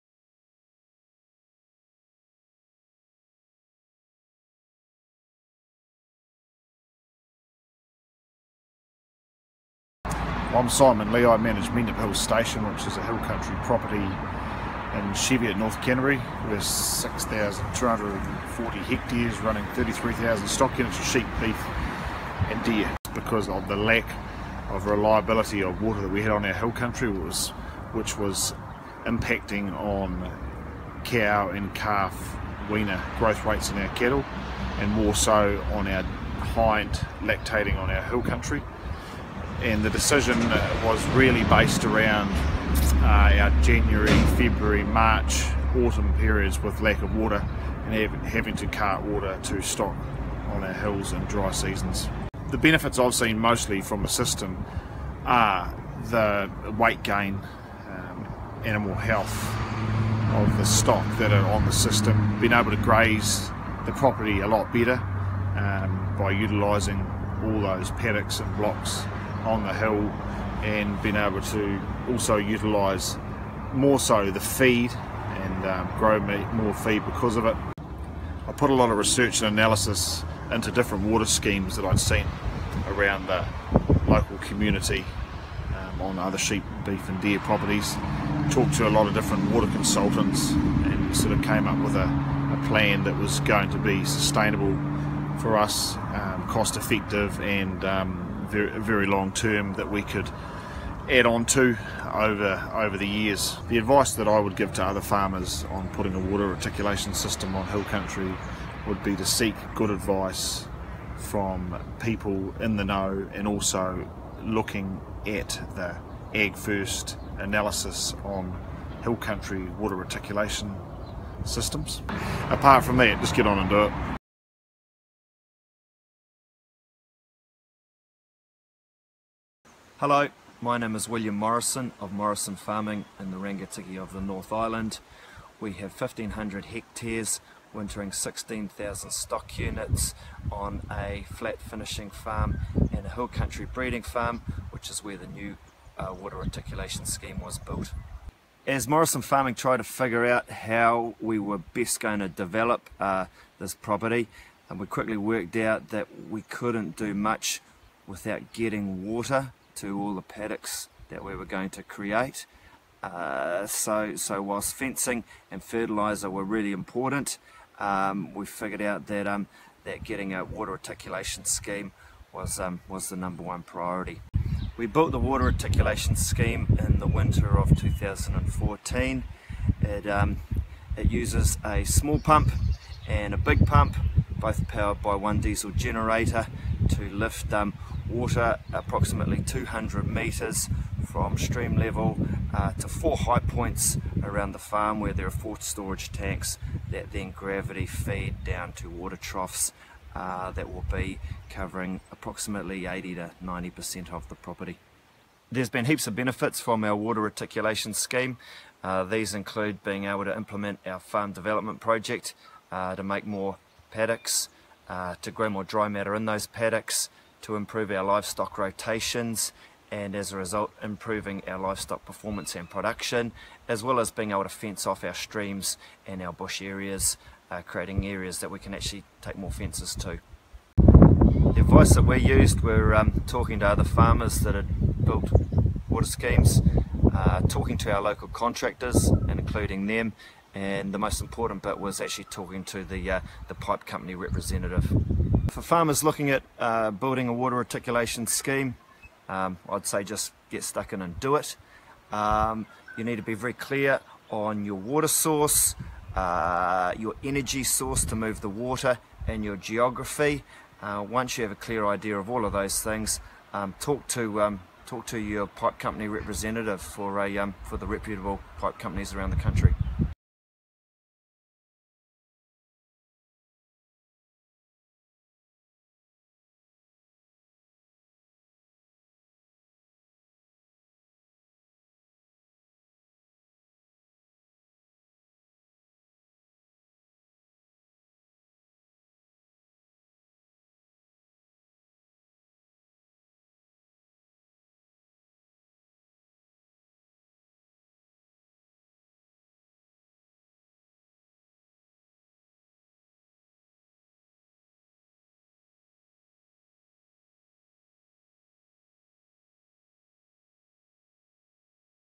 Case studies: Stock Water Reticulation Study
Two case studies from farmers who took part in the Economic Evaluation of Stock Water Reticulation on Hill Country study.

The study was completed by AgFirst in 2016 and published in 2017.

The report was funded by the Ministry for Primary Industries, the Ministry of Business Innovation and Employment, Te Puni Kokiri, and Beef + Lamb New Zealand.